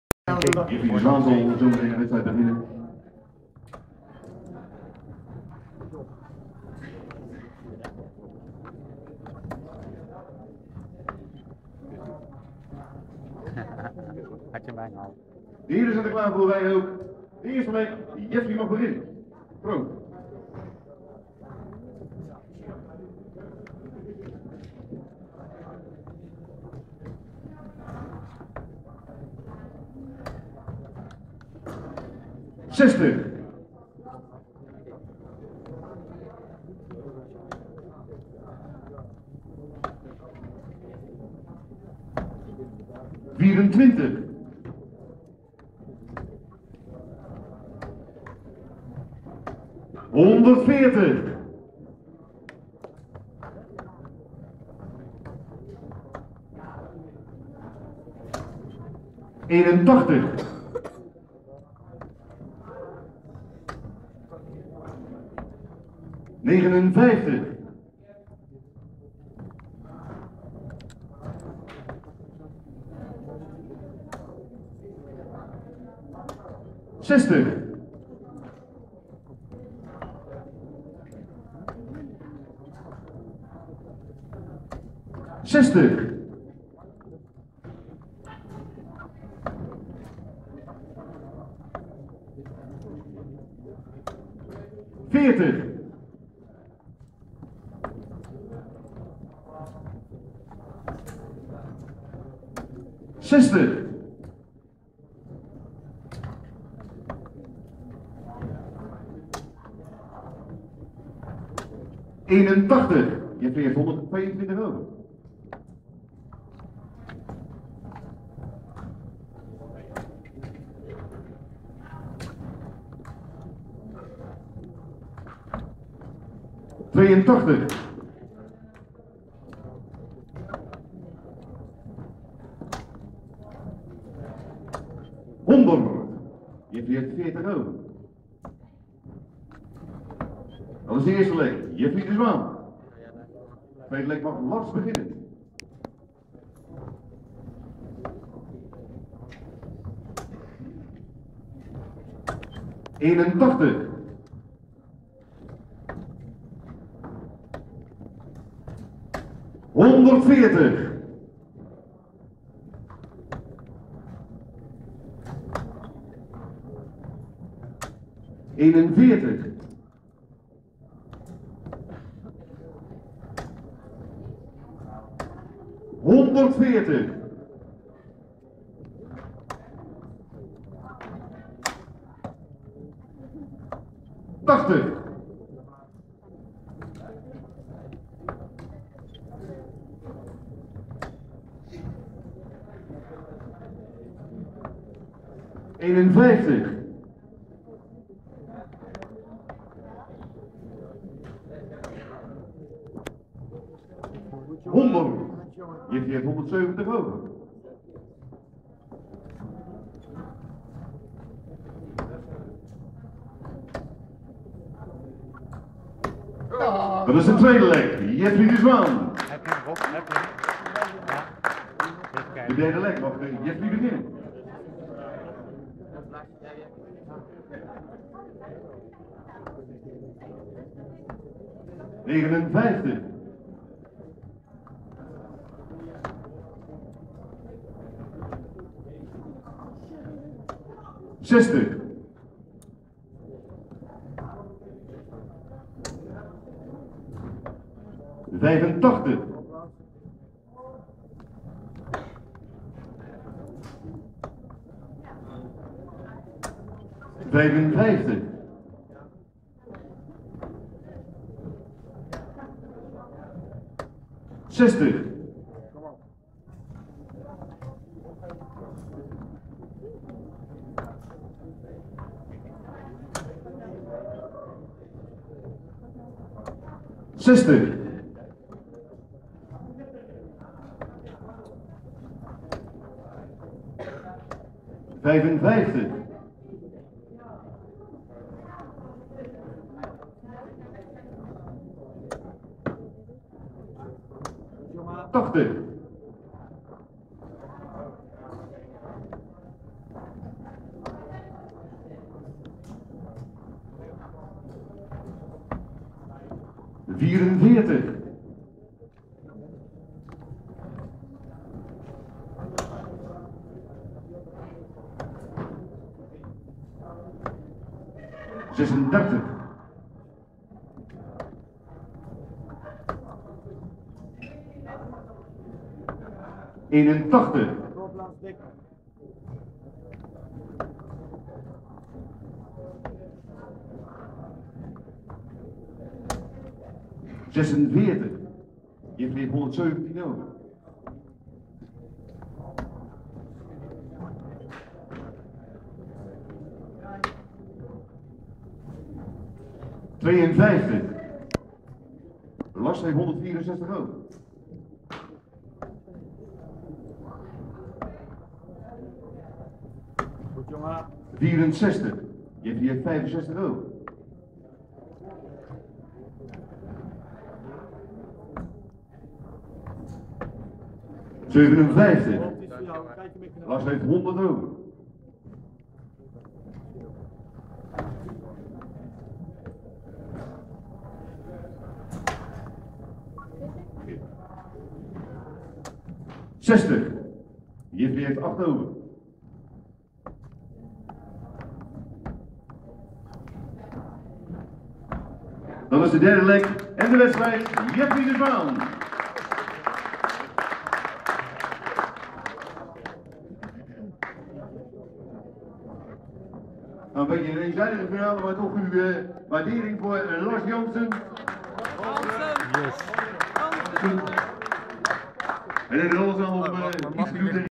Finale aspiranten Bossche NDB ranking 2010 Jeffrey de Zwaan 170 finish
Finale aspiranten Bossche NDB ranking 2010: Jeffrey de Zwaan wint met 3-0 van Lars Fransen en gooit in de tweede leg zijn derde 170 finish van het seizoen dat hij door deze overwinning afsluit als Nederlands ranking kampioen.